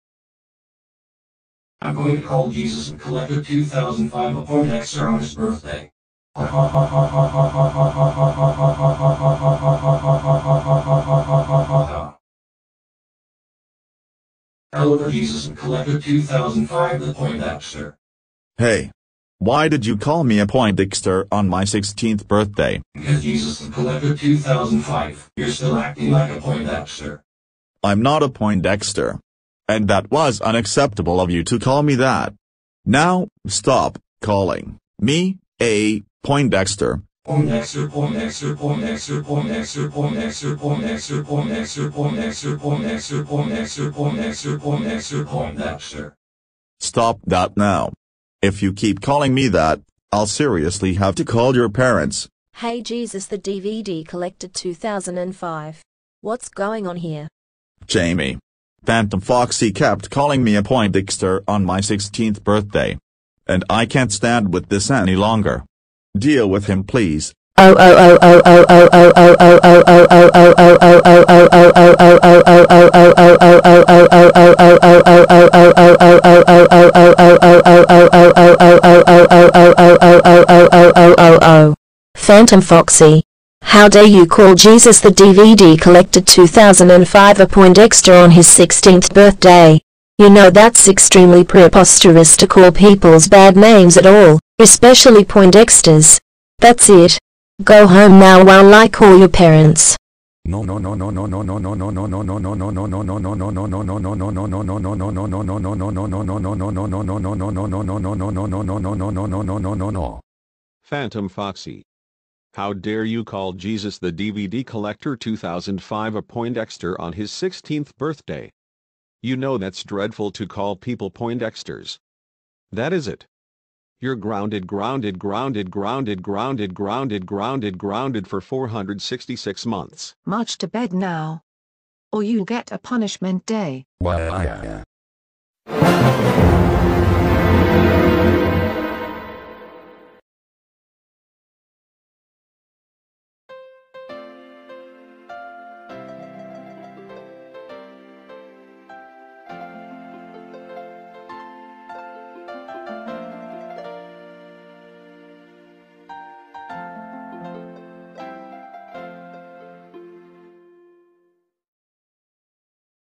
Phantom Foxy Calls Jesus TheDVDCollector2005 A Poindexter On His 16th Birthday/Grounded (UPDATED)
Update: Added Kiera And Ahsoka To The Party